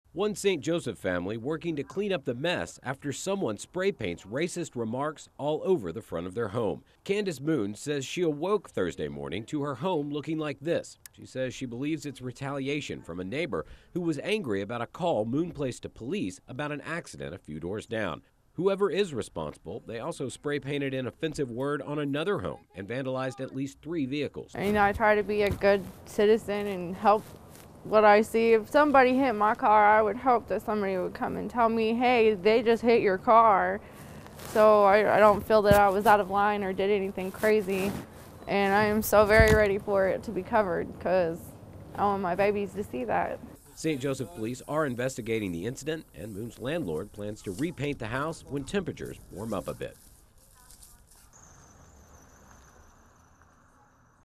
Home vandalized with racist graffiti
One St. Joseph family is working to clean up the mess after someone spray painted racist remarks all over the front of their home. (Original Air Date: 12/04/14; 5:00 p.m.)